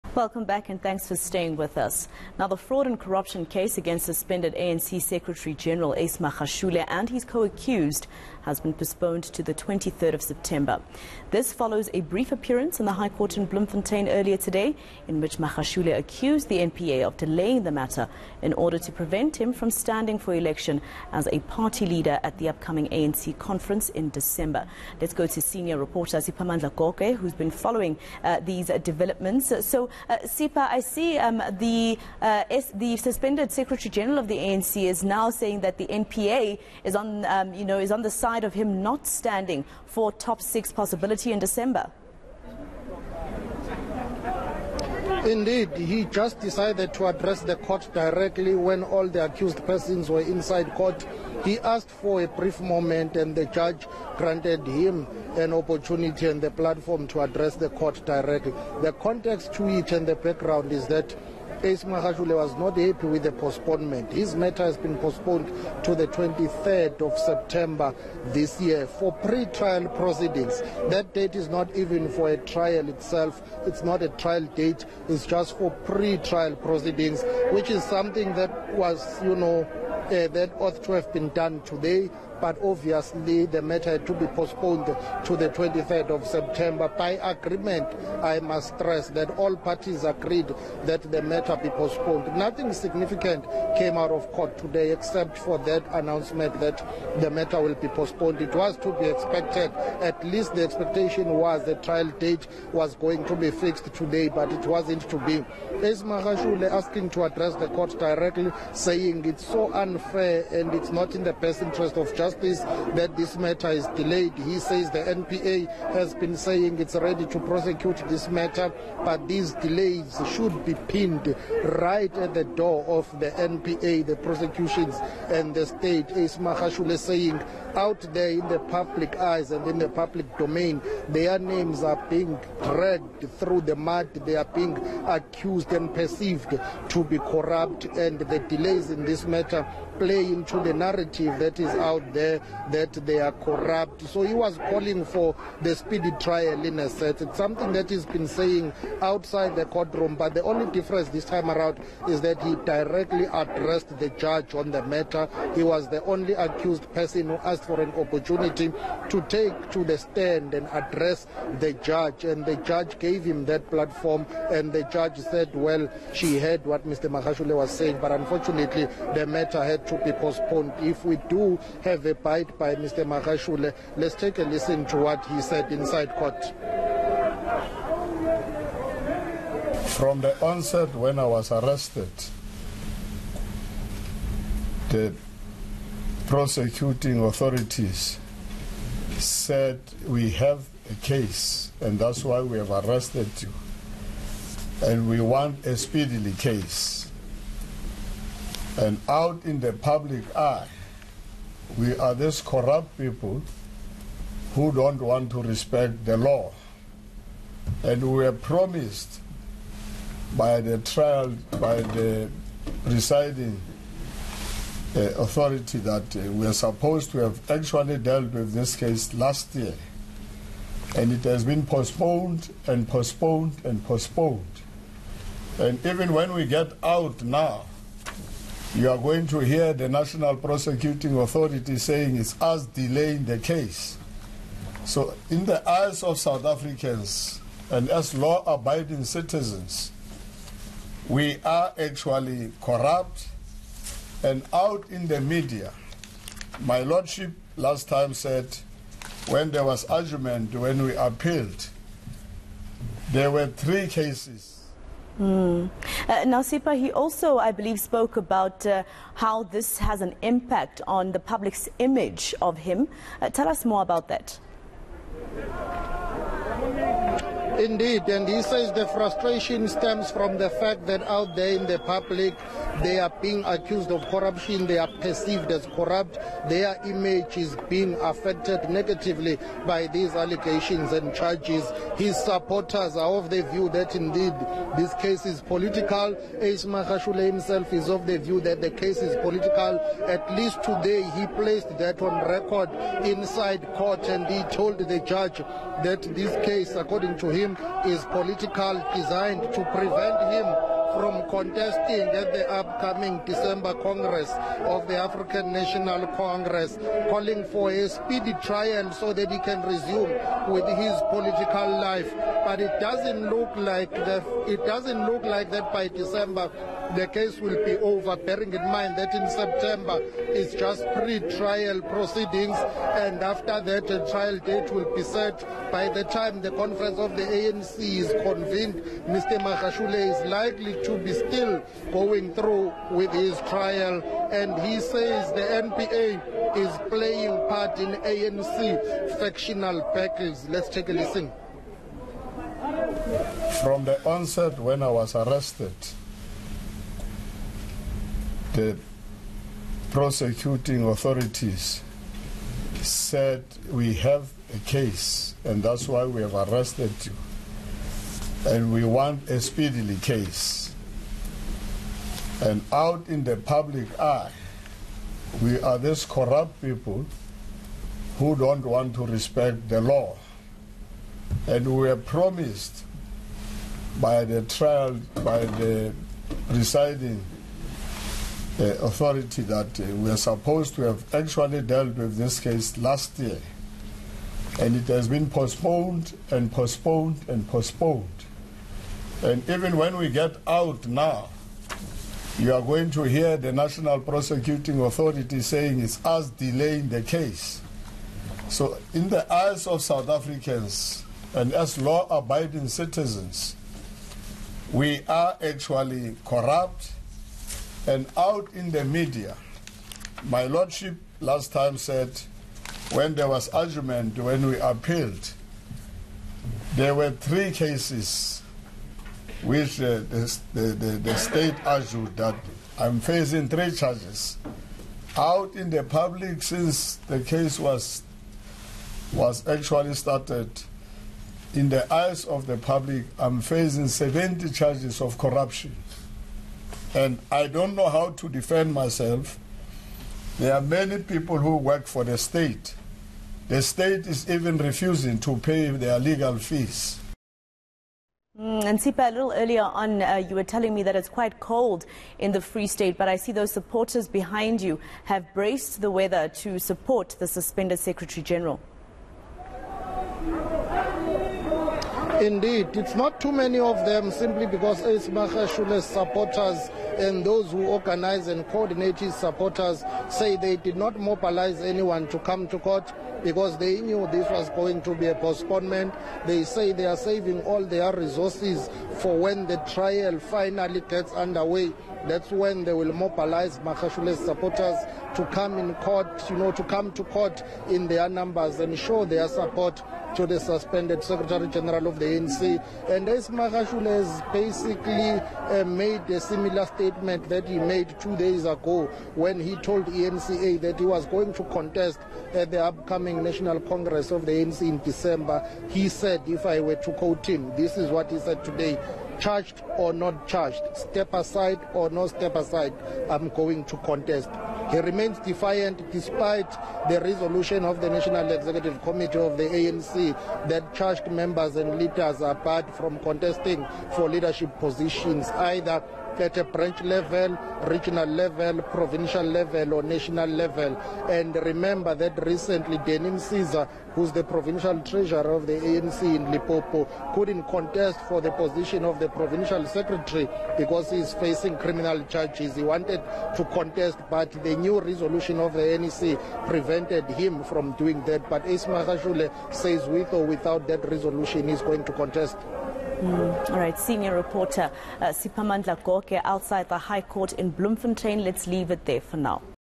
Magashule corruption case postponed to September
The fraud and corruption case against suspended ANC Secretary-General Ace Magashule and his co-accused has been Postponed to 23 September 2022. eNCA's Siphamandla Goge is following this story. Courtesy